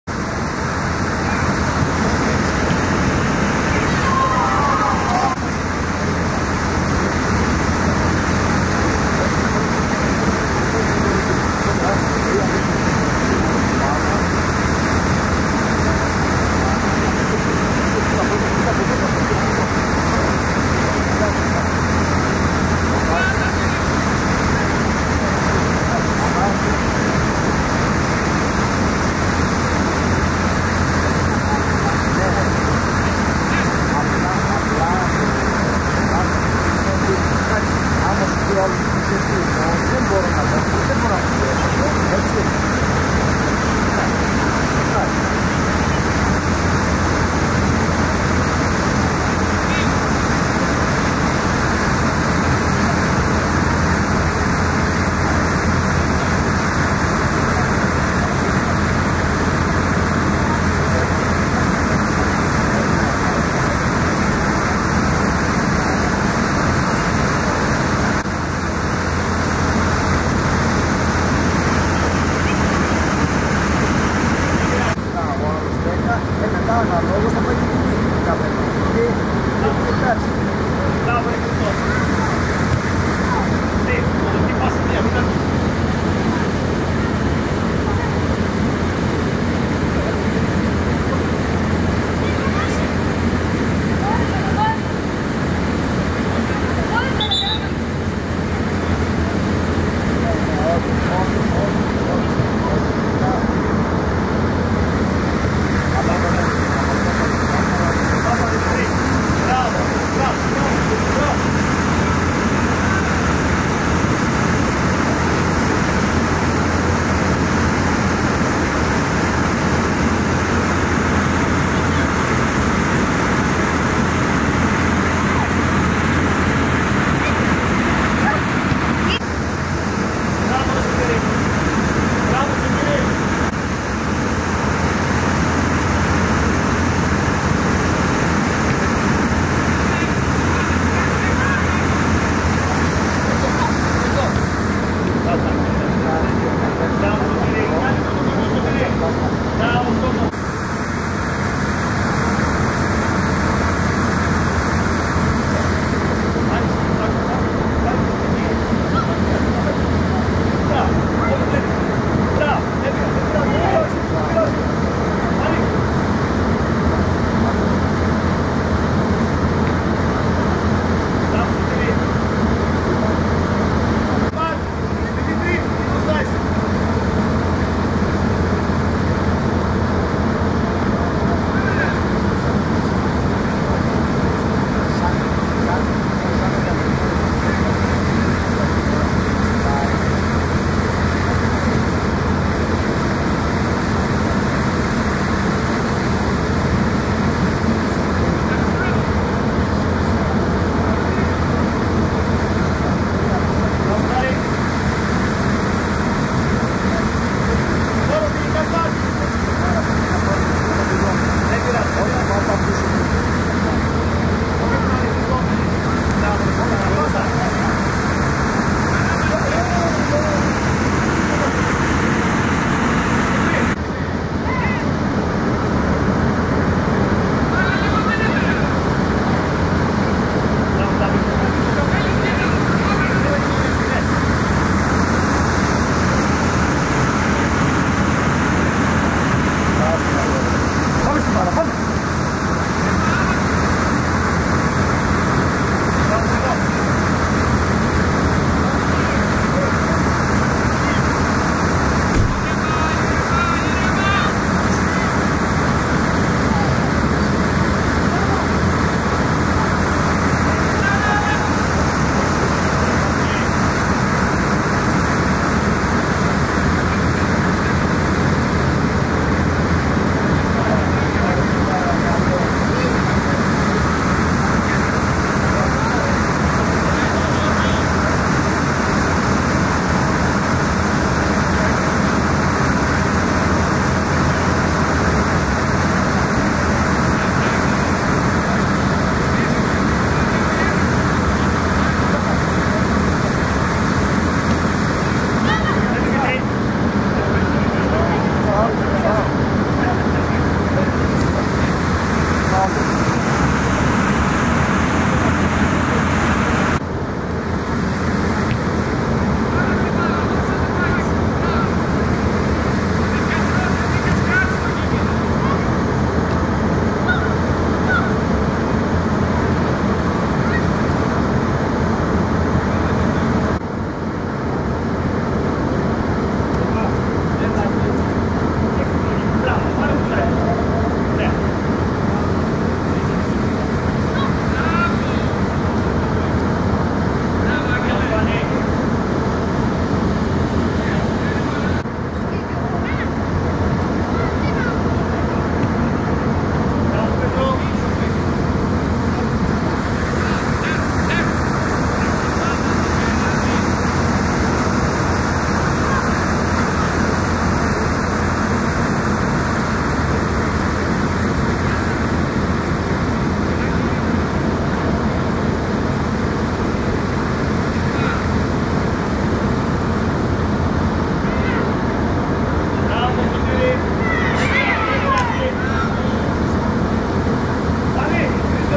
Κ12Γ Άτλας Δ - Αστέρας Ι. Γ (9η αγωνιστική/2023-2024)
Στιγμιότυπα από το παιχνίδι πρωταθλήματος Κ12Γ.